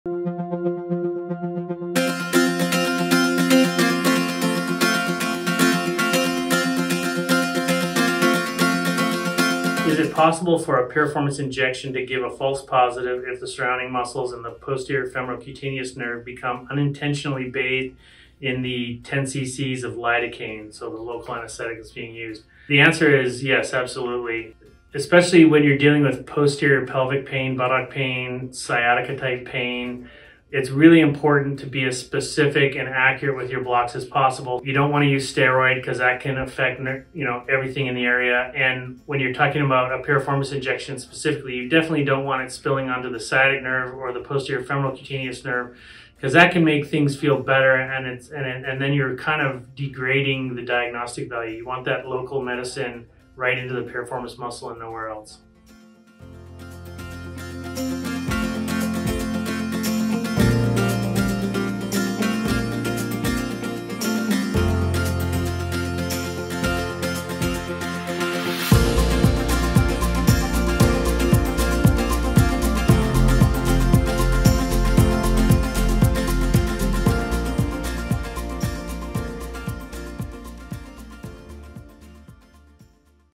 Ask Dr. Tollestrup: Location of Nerve Block for a Piriformis Injection
In this segment of Ask Dr. Tollestrup he addresses the importance of accuracy in the nerve block to diagnose Piriformis Syndrome.
If the injection is off even by a little it can impact the value of the diagnosis.
Join us Wednesday October 26th at 12 pm PST for a Facebook Live session where Dr. Tollestrup addresses many of the questions submitted.

Dr. Tim Tollestrup responds

Question: “Is it possible for a piriformis injection to give a false positive if the surrounding muscles and the posterior femoral cutaneous nerve become unintentionally bathed in the 10 ccs of Lidocaine (the local anesthetic being used)?”

Answer: “The answer is yes especially when you're dealing with posterior pelvic pain buttock pain and sciatica type pain it's important to be as specific and accurate with your blocks as possible.
You don't want to use steroid because that can affect you know everything in the area and when you're talking about a piriformis injection specifically, you don’t want it spilling onto the sciatic nerve or the posterior femoral cutaneous nerve because that can make things feel better. Then you're degrading the diagnostic value.
You want that local medicine right into the piriformis muscle and nowhere else.”